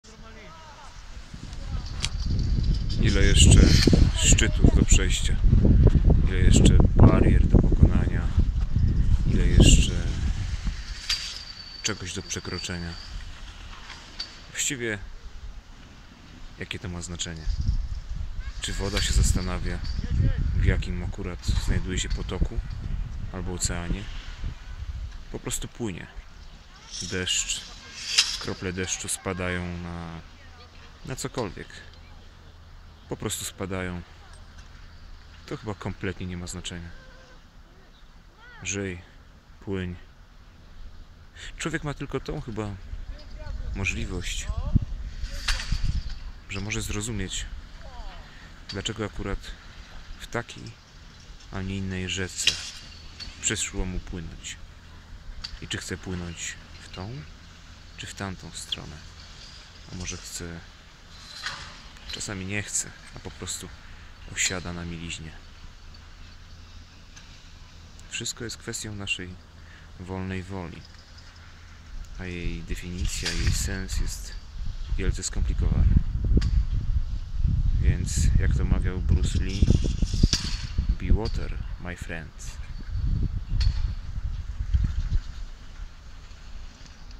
BE WATER, MY FRIEND... 2016.01.27
Zjeżdżałem, zjeżdżałem... i w końcu przysiadłem na stoku... Śnieg na Czarnej parował i coraz więcej było ziemistych prześwitów, a mimo to orczyk wciąż chodził i chodził...
Ile jeszcze szczytów do pokonania... Ile jeszcze barier do przejścia.
Refleksje przychodzą do Ciebie w każdym momencie - musisz je tylko wyłowić z rwącej rzeki uczuć i myśli... Po co? Ano po to, żeby się ze sobą dogadać, być ze sobą w kontakcie... i słuchać...